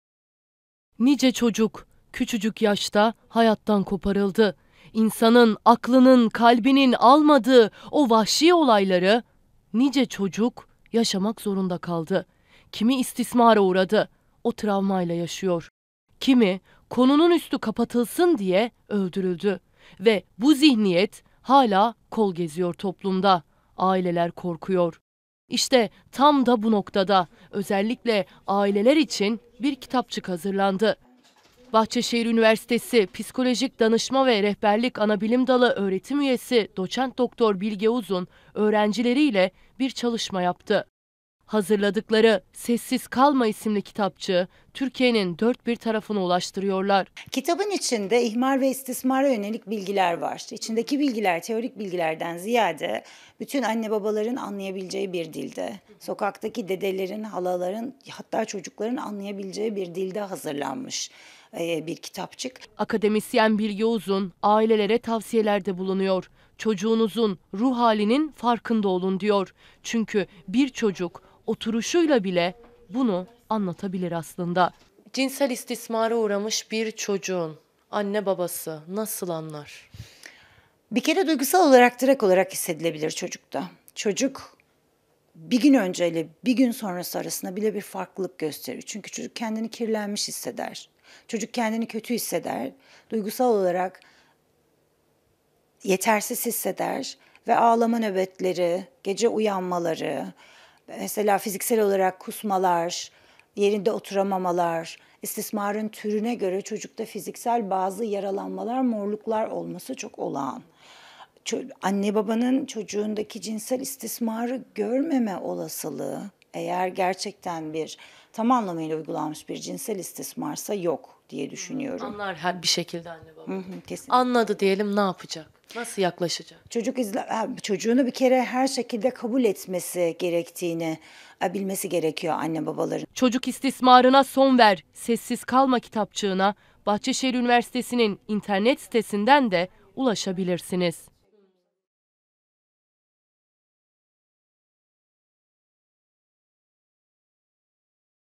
Çocuk istismarı kitapçığı hazırlandı: Sessiz Kalma
Son günlerin gündem başlıklarından biri çocuk istismarı. Bahçeşehir Üniversitesi öğrencileri de çocuk istismarına sessiz kalmadı. Tam da bu isimle "Sessiz Kalma" adında bir kitapçık hazırladı. Türkiye'nin dört bir yanına ulaştırdıkları o kitapçık anne-babalara rehber olacak. CNN TÜRK muhabiri Fulya Öztürk, detayları Bahçeşehir Üniversitesi PDR Anabilim Dalı Öğretim Üyesi Doç. Dr. Bilge Uzun'la konuştu.